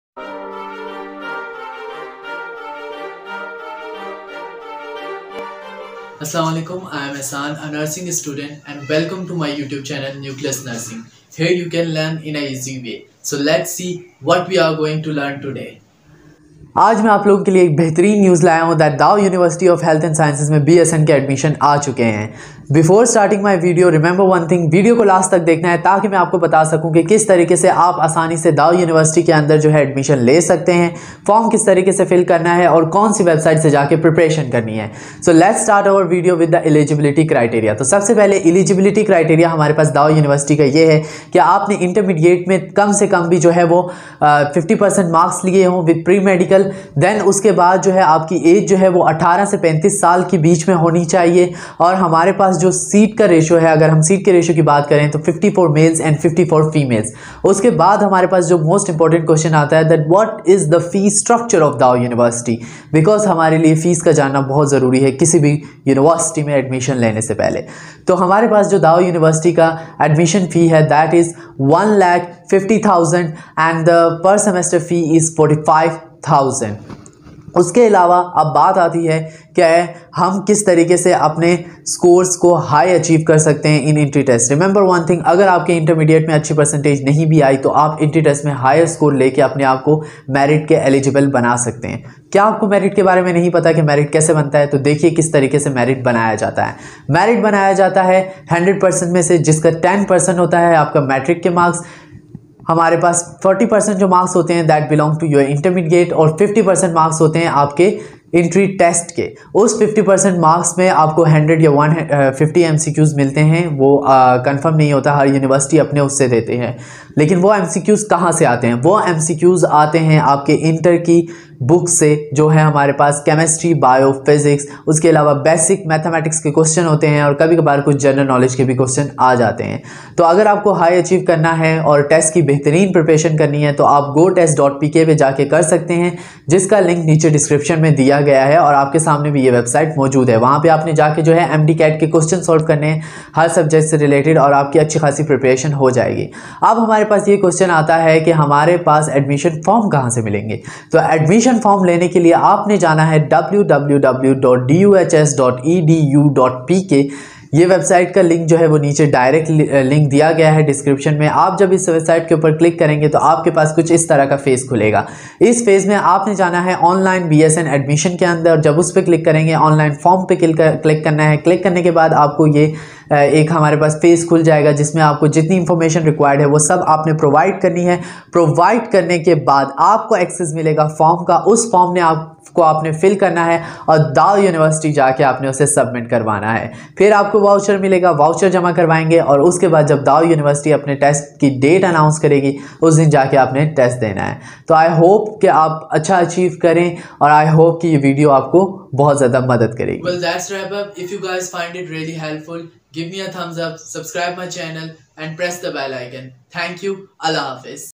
How to apply DOW university for BScN in Urdu || fee structure, entry test, admission forms & merit
Admissions has been open at Dow university of health and sciences. So if you have the domicile of sindh you can apply here, and get yourself on merit.

To know more information watch the complete video above.